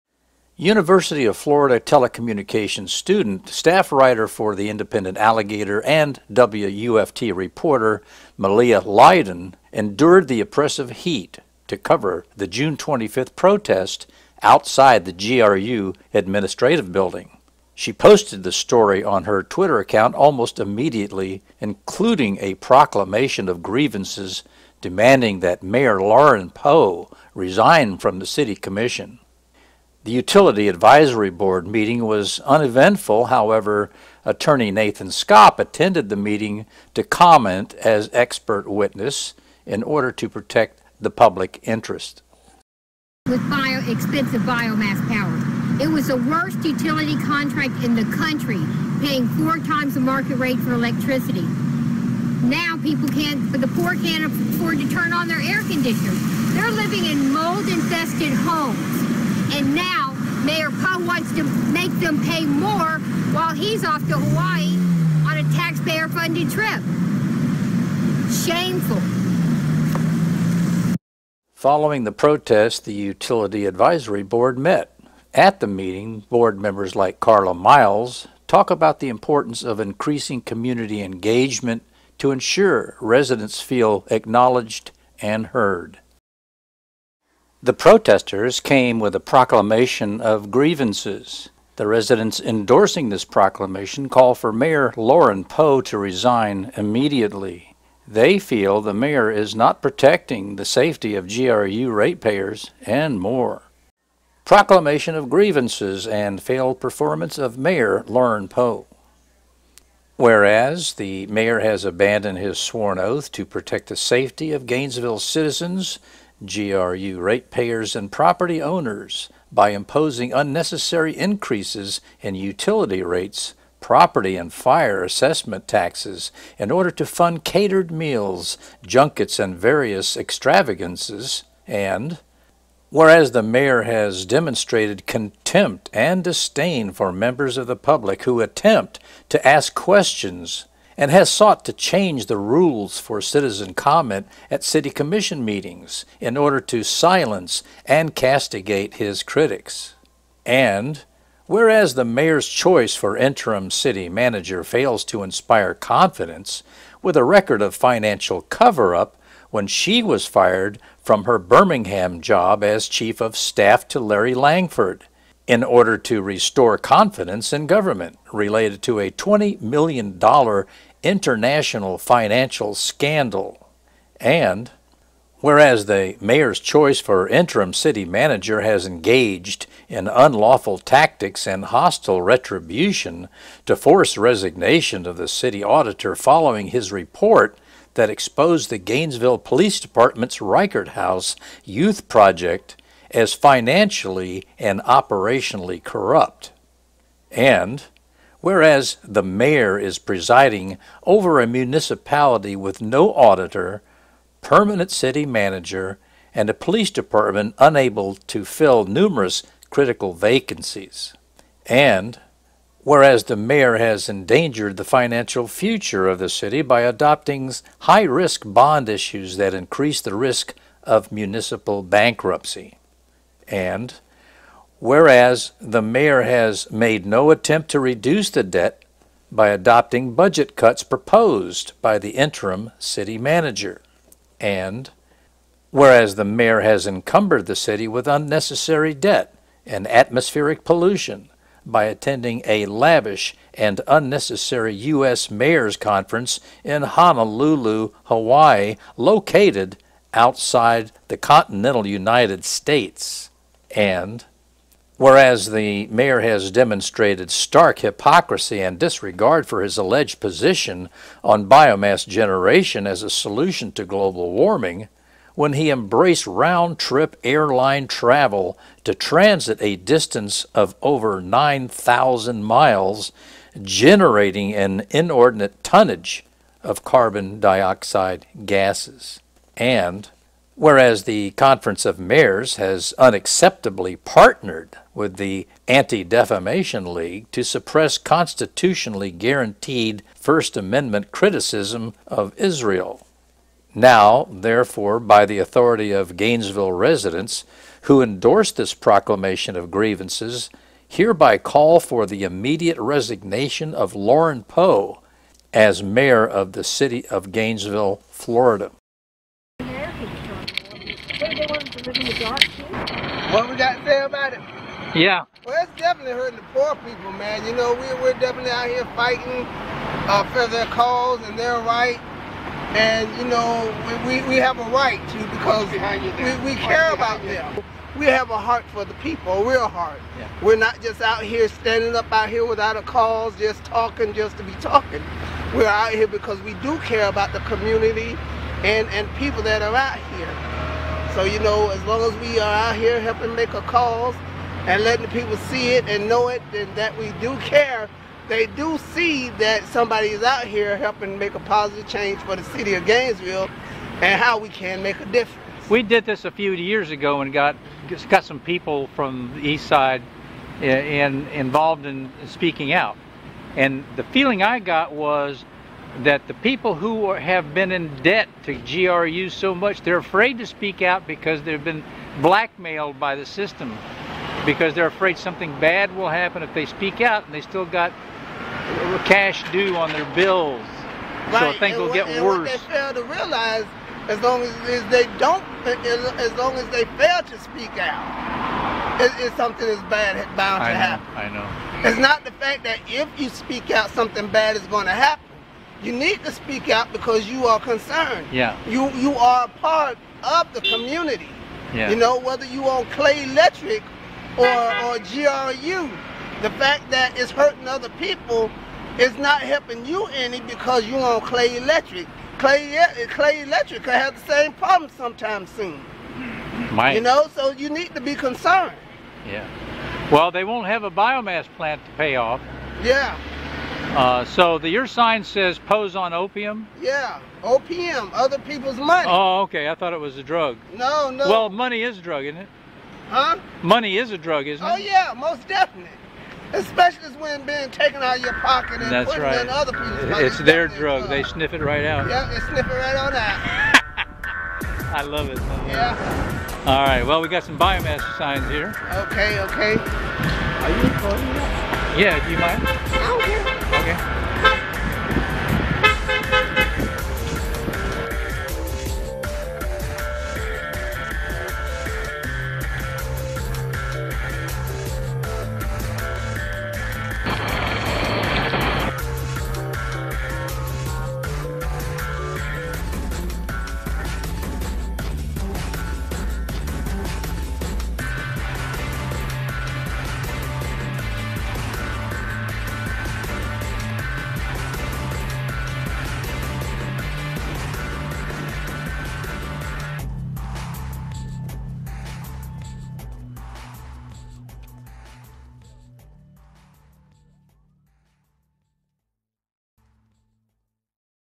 Gainesville Protest and Proclamation Demanding Resignation of Mayor Lauren Poe
REICHERT HOUSE COVERUP - MOTIVE TO OUST AUDITOR
Whereas the Mayor's choice for interim City manager has engaged in unlawful tactics and hostile retribution to force resignation of the City auditor following his report that exposed the Gainesville Police Department's Reichert House youth project as financially and operationally corrupt ;

TIP OF THE ICEBERG?
Sex act on Reichert House bus not reported to Gainesville Police for 10 days